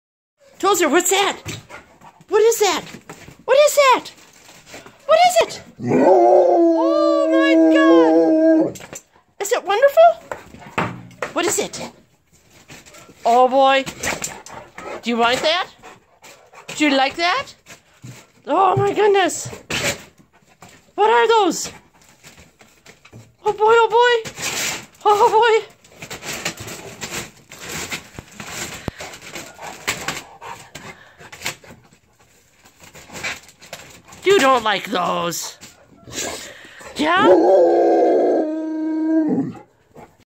Dogs love carrots, too!
120-lb Chesapeake Bay Retriever is surprised with a 40-lb bag of carrots.